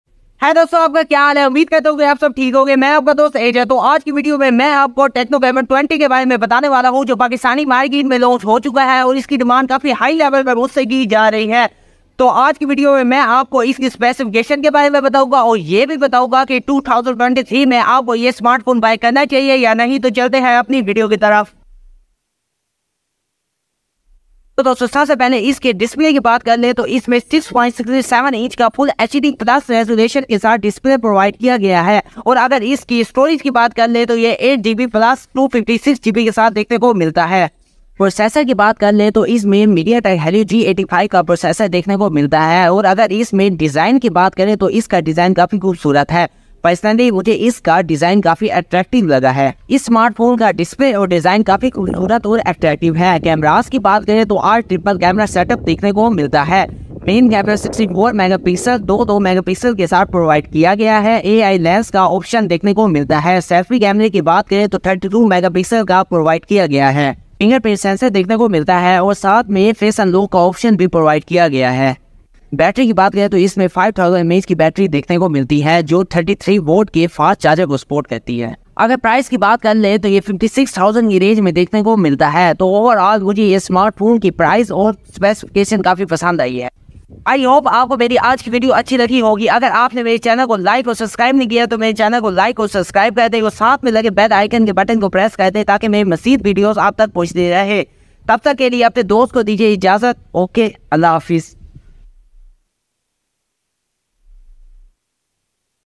Tecno Camon 20 Review In 2023 | Best Budget Phone Tecno Camon 20 | Camon 20 Specs, Price and Date
Welcome Dear Viewers If you're in the market for a new smartphone, you might want to check out the Tecno Camon 20. In this video, we're giving you a comprehensive review of this phone. Yes, that's right - it's still a great option in 2023! We'll go over all the features and specs that make the Camon 20 stand out from the competition, including its impressive camera system and long battery life. Plus, we'll give you our honest opinion on whether or not it's worth buying today.

Watch My More Informative Videos